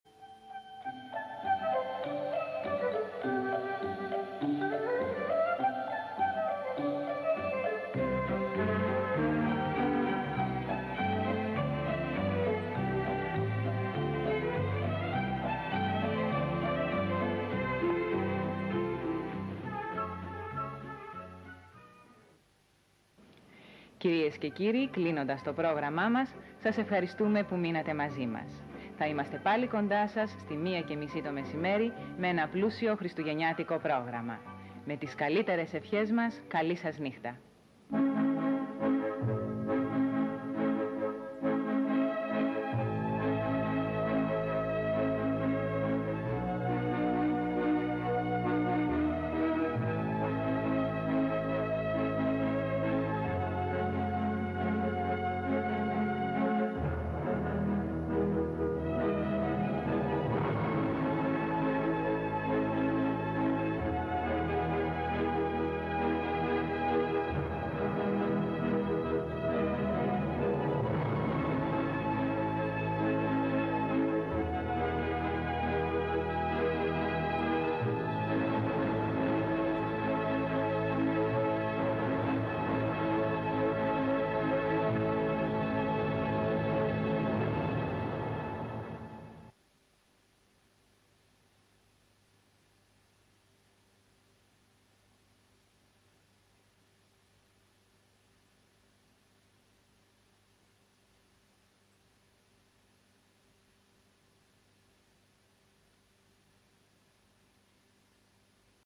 ΔΗΜΗΤΡΑ ΠΑΠΑΔΗΜΑ MEGA
Το τέλος του προγράμματος του MEGA με την Δήμητρα Παπαδήμα που μας λέει καληνύχτα και όνειρα γλυκά από την συχνότητα του MEGA. Μεταδόθηκε Δευτέρα 25 Δεκεμβρίου 1989.